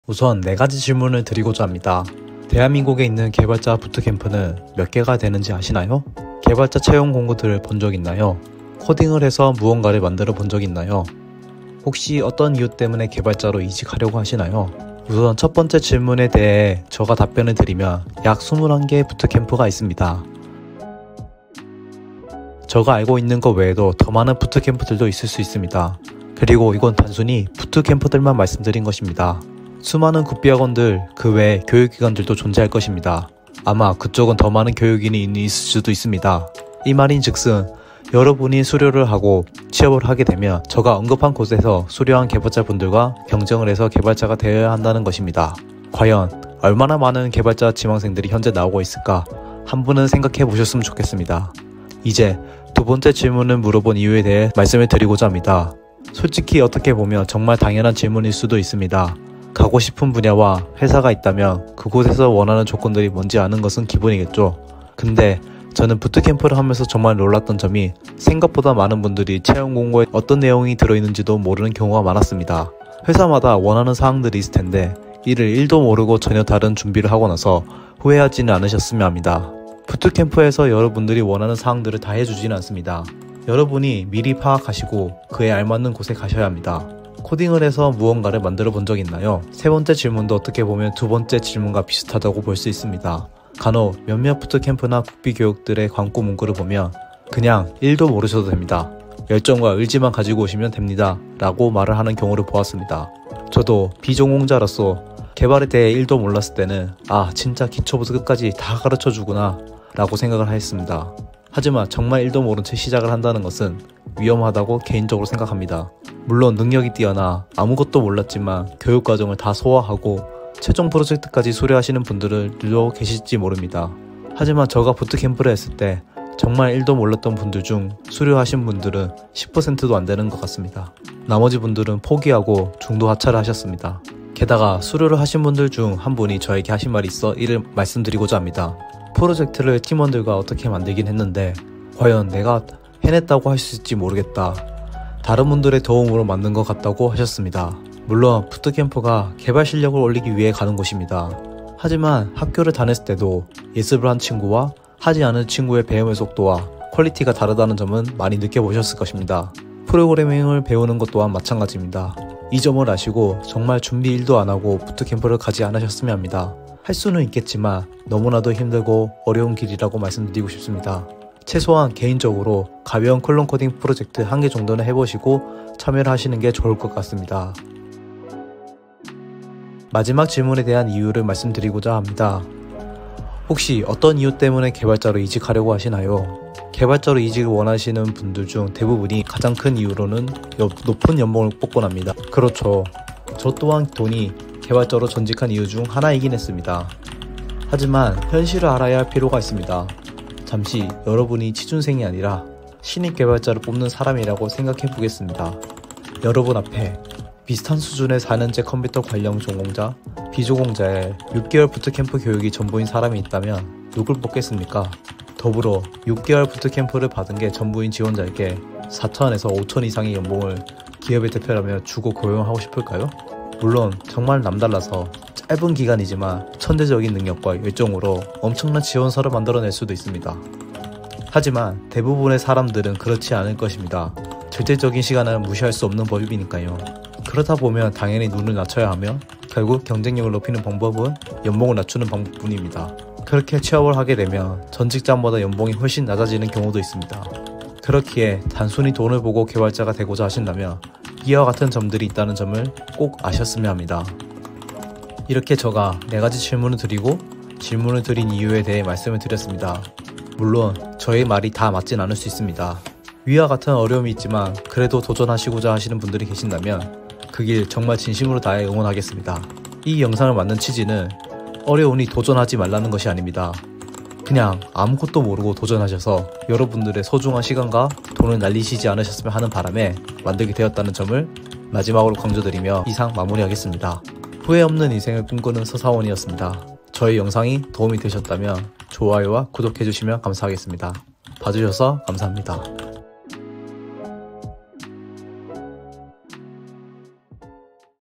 이거 안보고 개발자 준비하겠다고?(비전공자라면 꼭 보세요)
비전공자 출신으로 신입개발자인 서사원입니다.
개발자를 준비하면서 겪은 일들과 현실적으로 일반적인 신입개발자의 위치에대해
한번 제가 느낀점들을 정리해보았습니다.

저또한 조금은(?) 개발교육 마케팅에 넘어간점이 있지만
앞으로 또 도전하실분들은 좀더 현실적인 정보들을 아셨으면 하는 바람으로
영상을 만들어보았습니다.

물론 저의 생각과 의견이 무조건 많지 않을  도 있음을 참고바랍니다.
(반박시 여러분의 의견이 무조건 맞습니다)